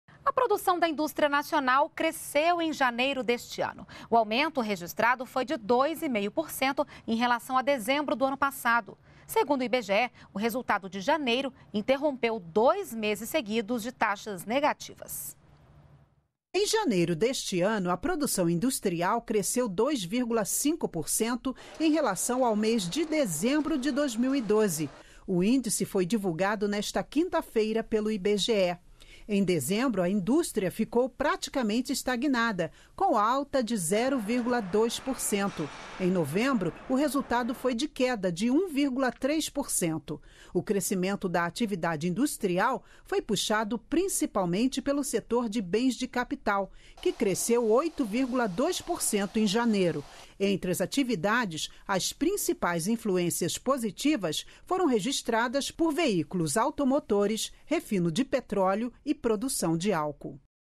Indústria nacional cresce 2,5%em janeiro deste ano em relação a dezembro de 2012
NBR NOTÍCIAS - 07.03.13: O Instituto Brasileiro de Geografia e Estatística (IBGE) aponta crescimento da produção da indústria nacional, em janeiro deste ano. O aumento registrado foi de 2,5% em relação a dezembro do passado. Segundo o IBGE, o resultado de janeiro interrompeu dois meses seguidos de taxas negativas.